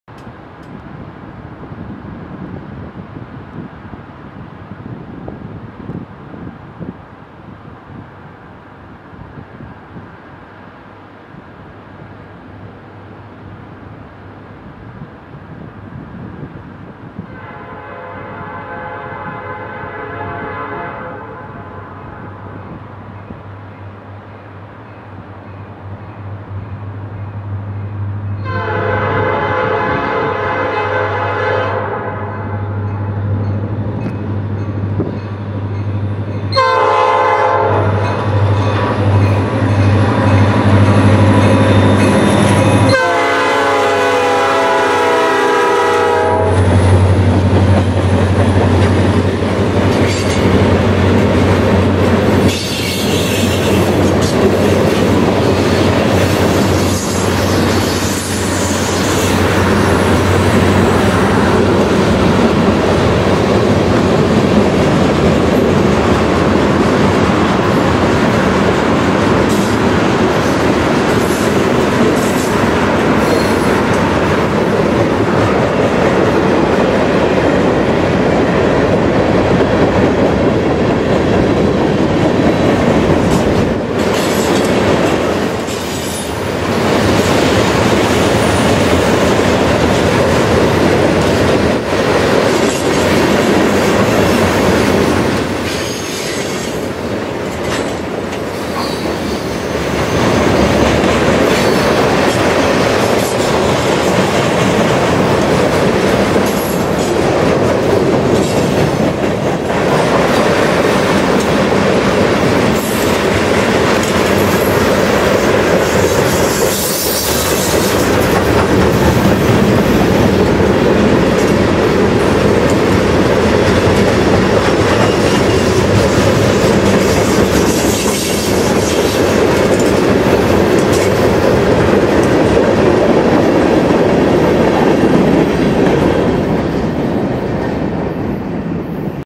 Emsworth 01: Black, White, and Clean All Over
Two well-polished NS units, which includes RP-E4C 708, leads my shortest of the day, a 66 car manifest through Emsworth.